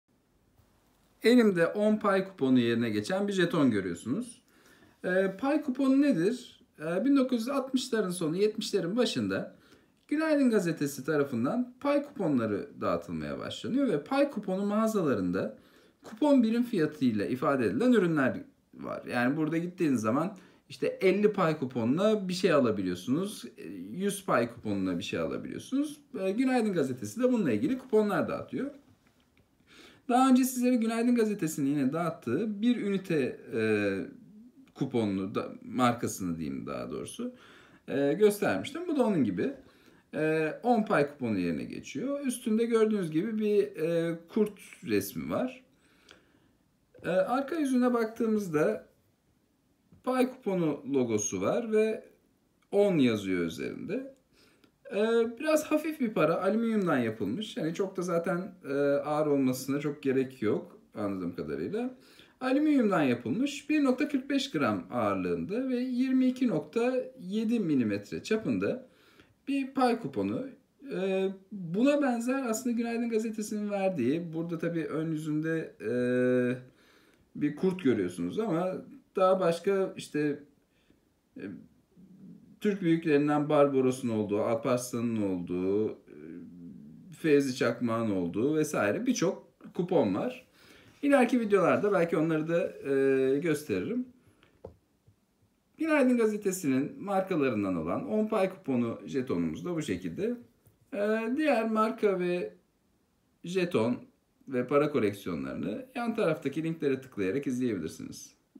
Günaydın Gazetesi On Pay Kuponu
Günaydın gazetesi tarafından 60lı ve 70li yıllarda verilen pay kuponları ile mağazalardan alışveriş yapılabiliyordu. Bu videoda on pay kuponu ile ilgili detaylı bilgi bulabilirsiniz.
Kullanımı yaygınlaşıp para yerine kullanılabilen bu kuponlar farklı madenlerden veya plastikten yapılmış olabiliyordu. Üzerinde farklı baskılar olan pay kuponları para koleksiyonu yapanlar tarafından da toplanmıştır.